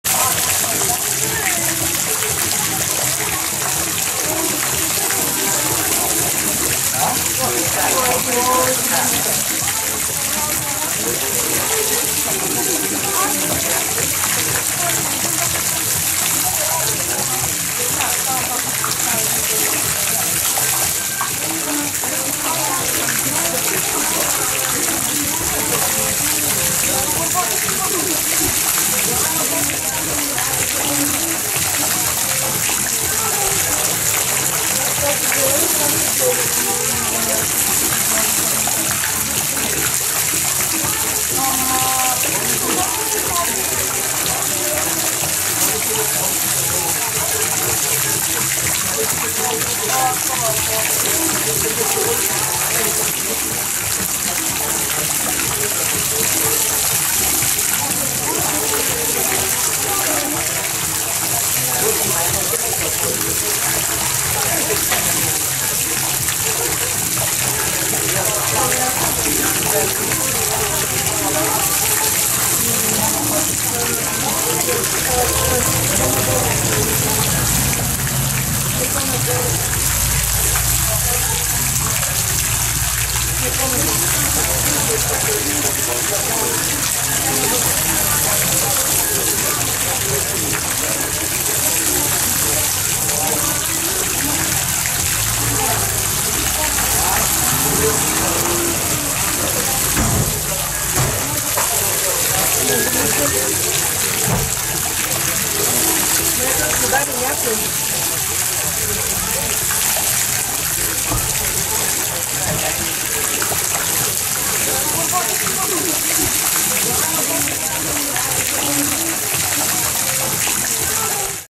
Turtles in a Fountain
Turtles in a Fountain enjoying the water and wandering around.